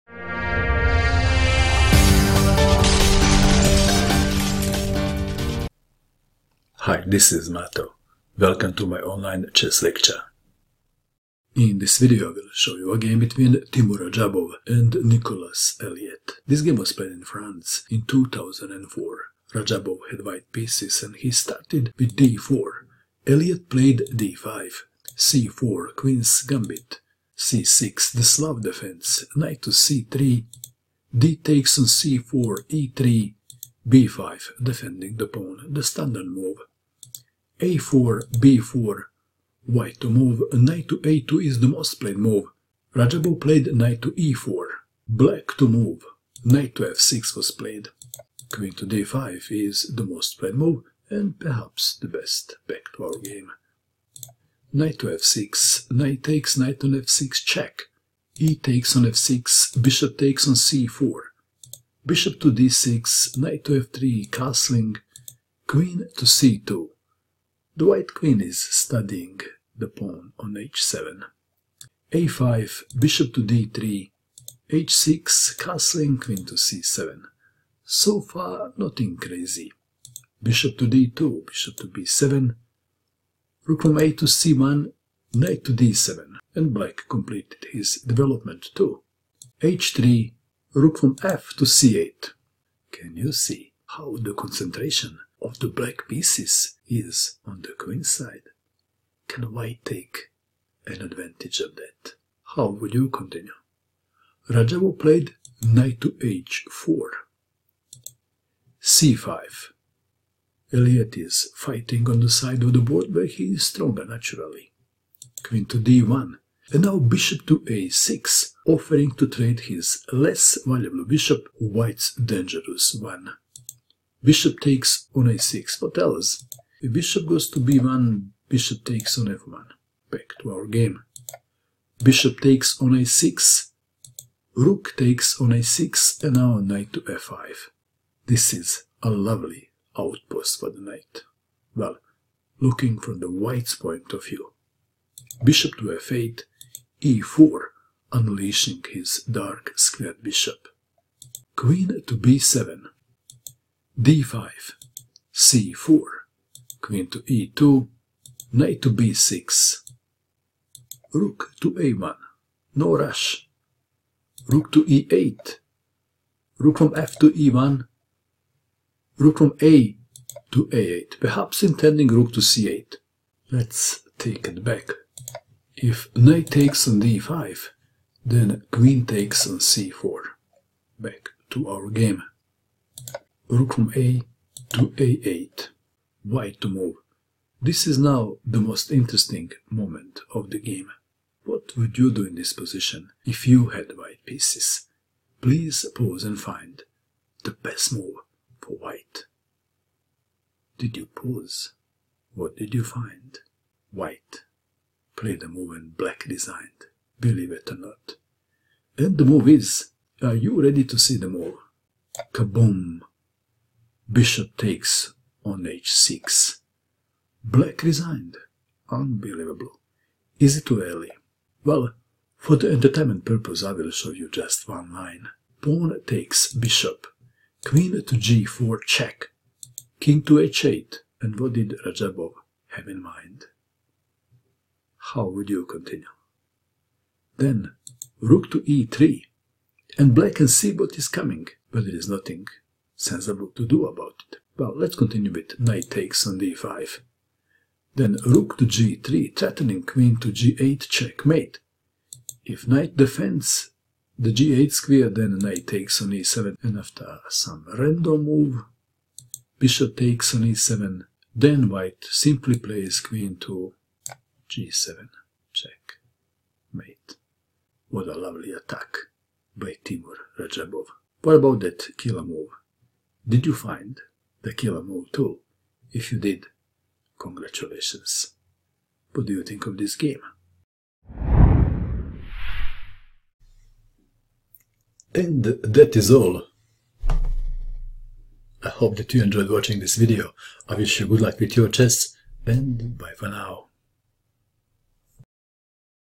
Crushing the slav defence and hunting the king | Teimour Radjabov vs Nicolas: Eliet France 2004
Teimour Radjabov vs Nicolas Eliet
FRA-chT Gp-A 2004, France, rd 3, Jan-30
Slav Defense: General (D10)  •  1-0
1.d4 d5 2.c4 c6 3.Nc3 dxc4 4.e3 b5 5.a4 b4 6.Ne4 Nf6 7.Nxf6+ exf6 8.Bxc4 Bd6 9.Nf3 0–0 10.Qc2 a5 11.Bd3 h6 12.0–0 Qc7 13.Bd2 Bb7 14.Rac1 Nd7 15.h3 Rfc8 16.Nh4 c5 17.Qd1 Ba6 18.Bxa6 Rxa6 19.Nf5 Bf8 20.e4 Qb7 21.d5 c4 22.Qe2 Nb6 23.Ra1 Re8 24.Rfe1 Raa8 25.Bxh6 1–0